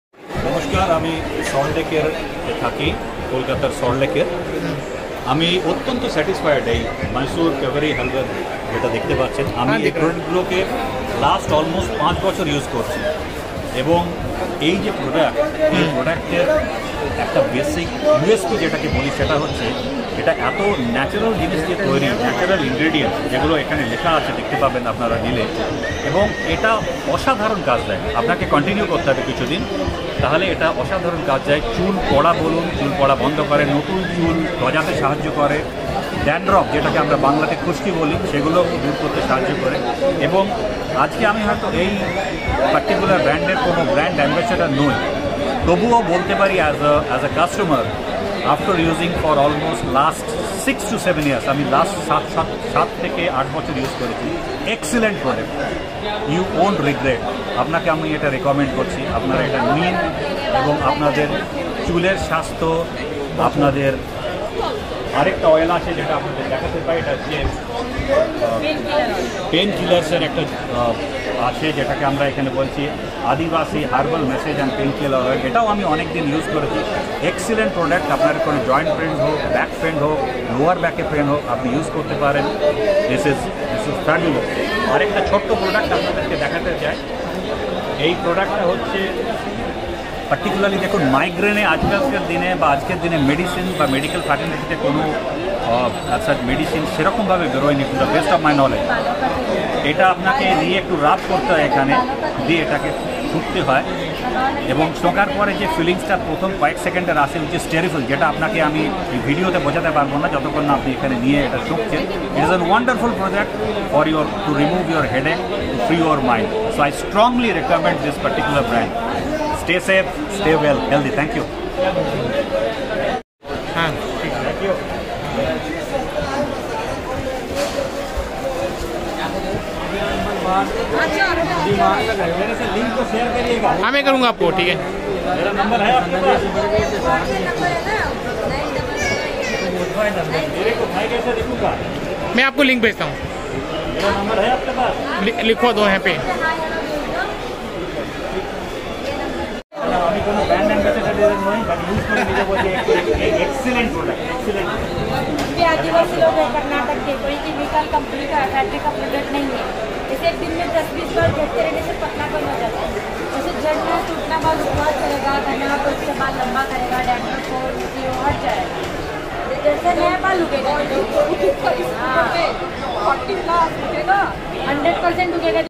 Mysore Kaveri herbal products adivasi hair care best hair oil best review in Kolkata customer
Mysore Kaveri herbal products adivasihaircare besthairoil best review in Kolkata customer

Mysore Kaveri herbal Adivasi Brungamalaka hair oil is basically made by pure Adivasi ayurvedic herbs which effectively prevent the hairs fall and help to grow long hairs with no side effect. ? This Hair Oil is used for Hair Problems like Hair Loss Dandruff Hair Growth Gray Hair New Hair Alopecia Dry Hair Spit Ends Dull Hair Frizzy Hair Use this Oil 2 to 3 times in a week. ? Used to make your Hair and to control Dandruff Apply the oil 2 to 3 times in a week Before Bath to stop the Dandruff And Healthy long Hair. ?